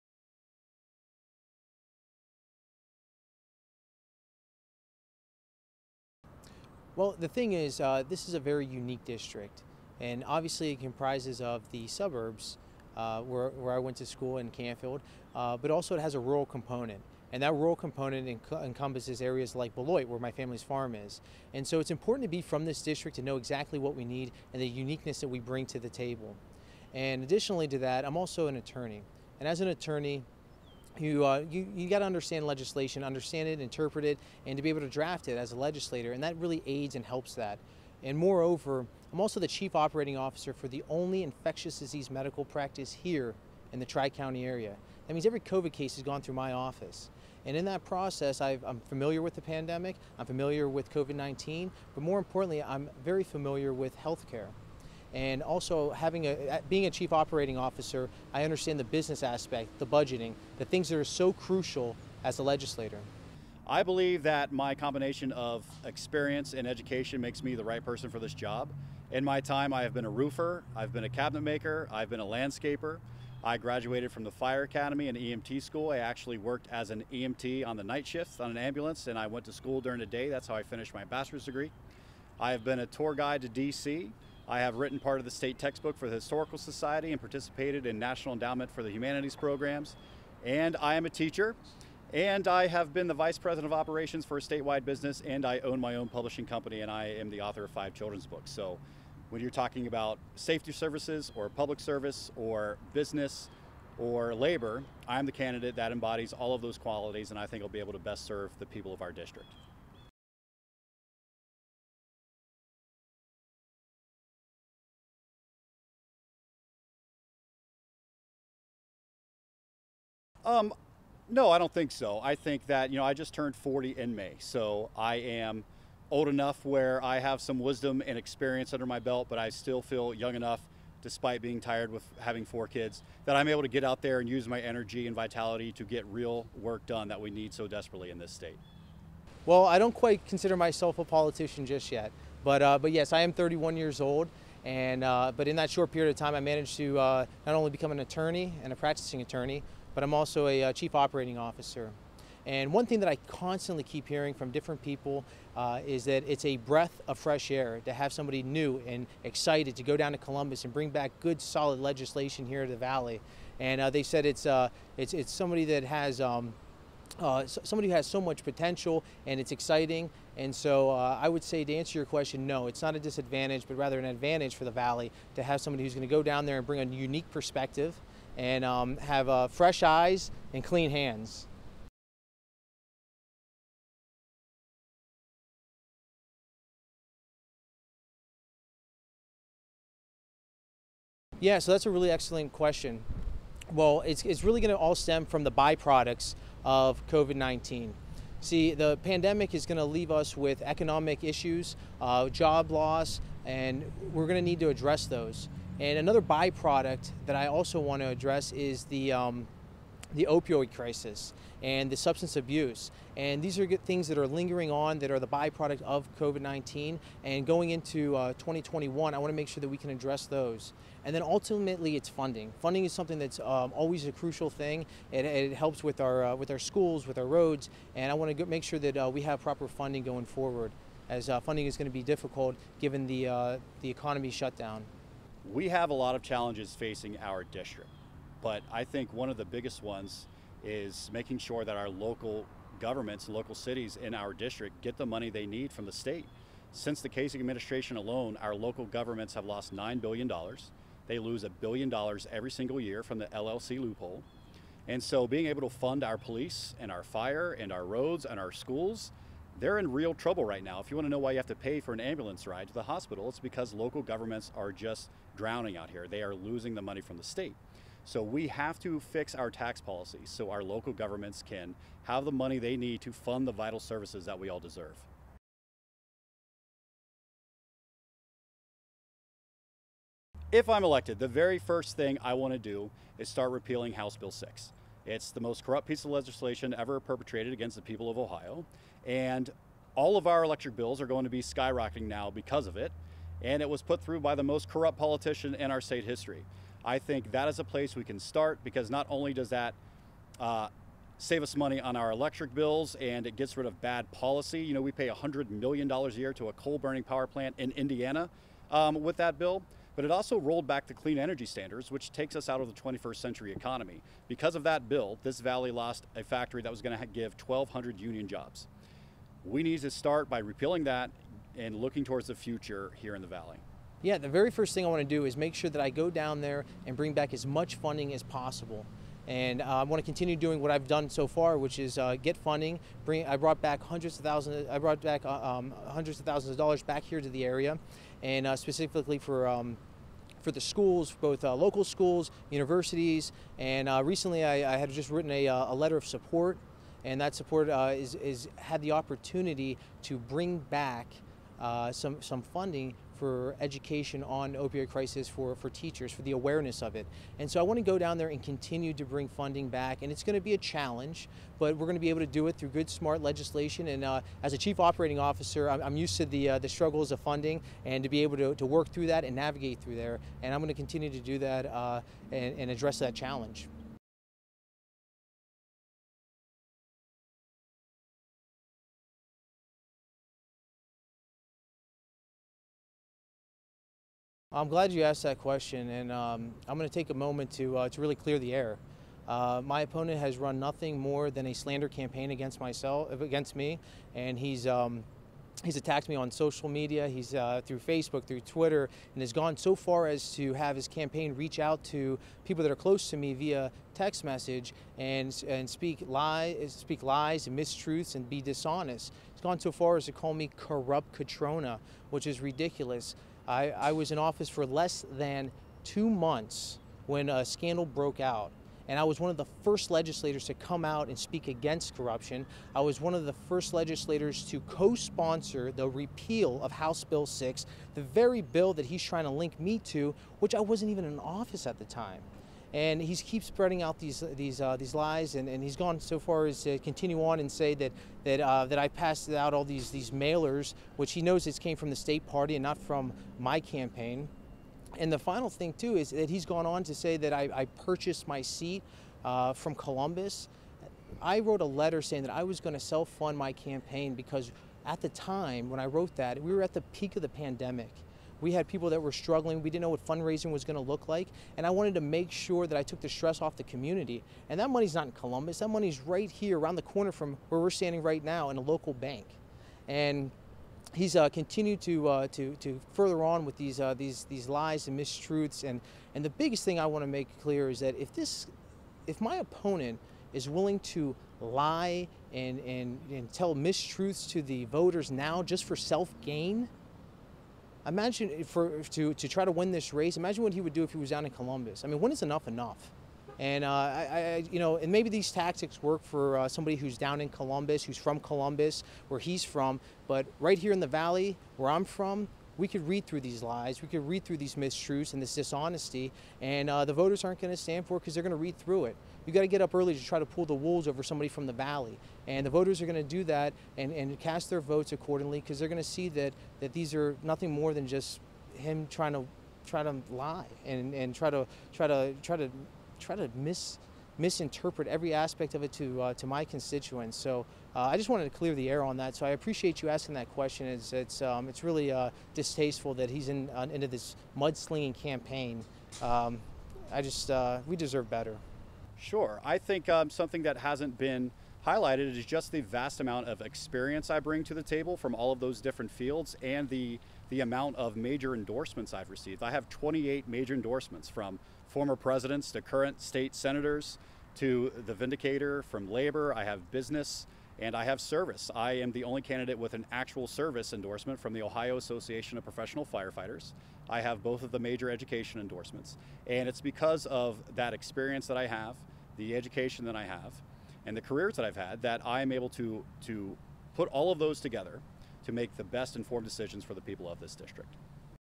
5 questions for 59th District Representative candidates Al Cutrona and Chris Stanley
Chris Stanley and Al Cutrona are running for representative of Ohio's 59th District